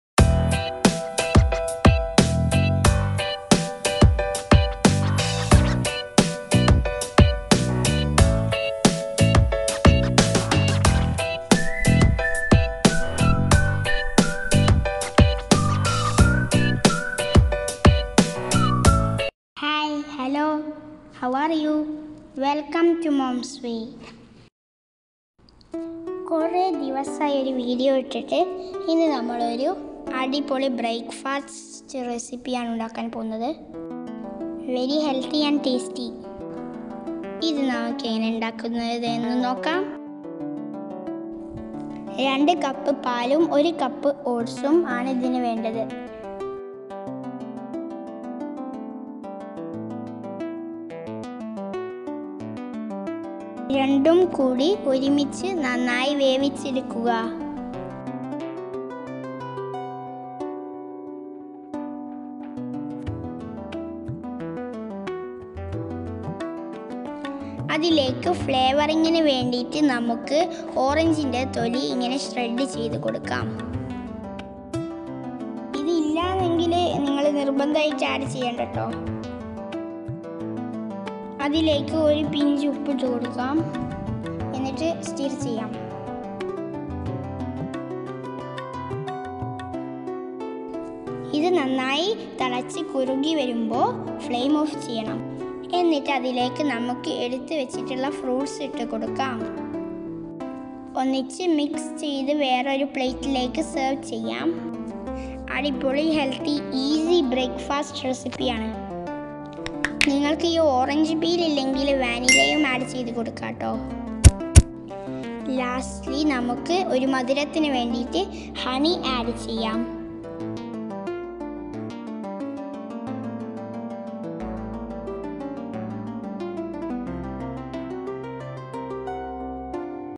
HOW TO COOK OATMEAL/Healthy & Easy Break fast Recipe/Healthy Oatmeal Recipes
Oatmeal is a breakfast food made from oats and liquid like water or milk. Many foods fall in and out of favor as health trends come and go. Not oatmeal. This whole-grain powerhouse has been packing serious nutrition and hearty flavor into breakfast for generations // healthy breakfast recipe// healthy meal/Healthy Oatmeal Recipes  HOW TO COOK OATMEAL/Healthy & Easy Break fast Recipe/Healthy Oatmeal Recipes